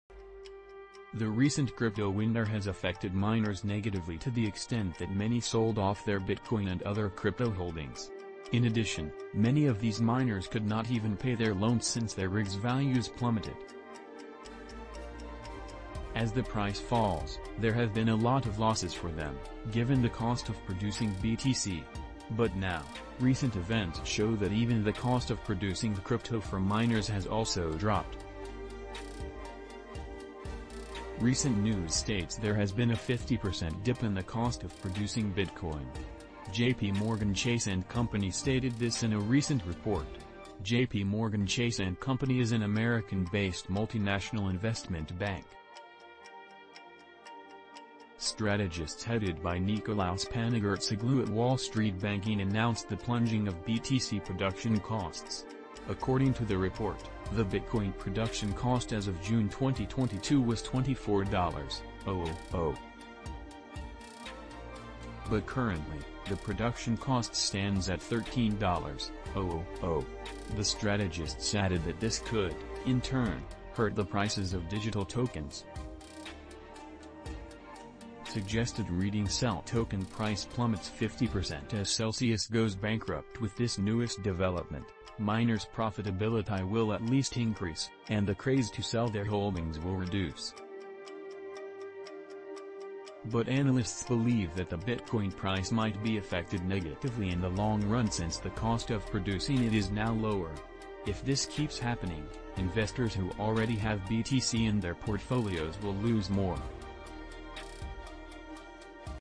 JP Morgan Claims Drop In Bitcoin Production Cost Is Negative For BTC Price
The recent crypto winter has affected miners negatively to the extent that many sold off their Bitcoin and other crypto holdings. In addition, many of these miners could not even pay their loans since their rigs’ values plummeted. As the price falls, there have been a lot of losses for them, given the cost of producing BTC. But now, recent events show that even the cost of producing the crypto for miners has also dropped. Recent news states there has been a 50% dip in the cost of producing Bitcoin. JP Morgan Chase & Co stated this in a recent report. JPMorgan Chase & Co is an American-based multinational investment bank. Strategists headed by Nikolaos Panigirtzoglou at Wall Street banking announced the plunging of BTC production costs. According to the report, the Bitcoin production cost as of June 2022 was $24,000. But currently, the production cost stands at $13,000. The strategists added that this could, in turn, hurt the prices of digital tokens. Suggested Reading | Avalanche Notches Solid Mid-Week Bounce – Can AVAX Sustain The Positive Noise? They also cited that the primary cause of the decline in the production cost can be traced to the limited use of electricity. This report was drawn from the Cambridge Bitcoin Electricity Consumption Index data. As per JPMorgan, this can also affect the price of Bitcoin, looking at the present bearish trend of the digital currency market. Bitcoin and the whole digital market have been facing a new phase of a bearish market. The event can be traced back to November 2021, after Bitcoin hit its ATH (all-time-high) of $69K. This occurrence has affected certain high-profile companies and blockchains. A prominent example to note is the crashing of the LUNA digital token, which was based on the Terra blockchain. Another instance includes the insolvency of Three Arrows Capital (3AC). Also, the information about the hiking rates of the Federal Reserve to fight inflation is another example to note. Drawing from the crypto market watch, the most significant digital token, BTC, has been fluctuating around the $20K mark. This is about 70% of the digital token’s price drop last year. The drastic price change in the Bitcoin price posed a high level of uneasiness in the minds of BTC miners. This high price crash was why many BTC miners sold off the digital asset. This was notable in the second quarter of this year. Suggested Reading | CEL Token Price Plummets 50% As Celsius Goes BankruptWith this newest development, miners’ profitability will at least increase, and the craze to sell their holdings will reduce. But analysts believe that the bitcoin price might be affected negatively in the long run since the cost of producing it is now lower. If this keeps happening, investors who already have BTC in their portfolios will lose more.